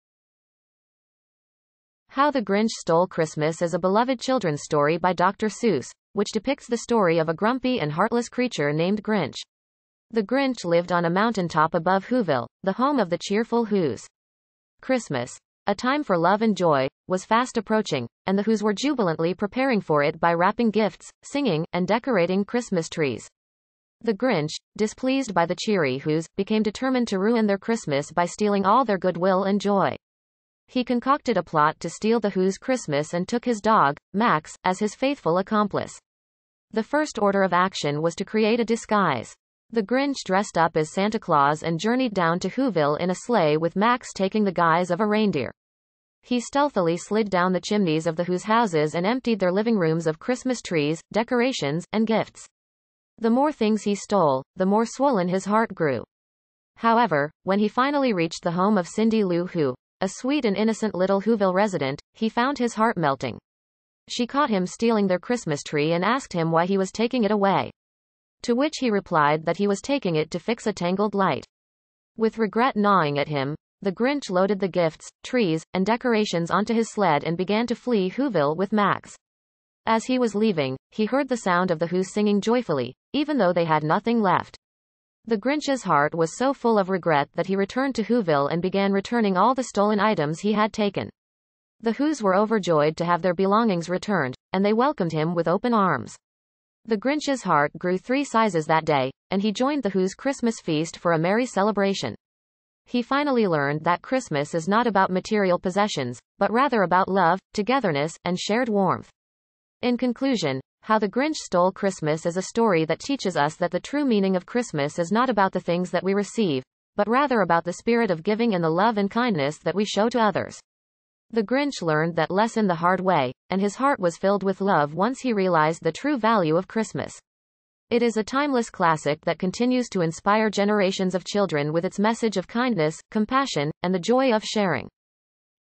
How the Grinch Stole Christmas
How the Grinch Stole Christmas is a beloved children's story by Dr. Seuss about a grumpy creature determined to ruin Christmas for the cheerful Whos of Whoville, but learns the true meaning of the holiday.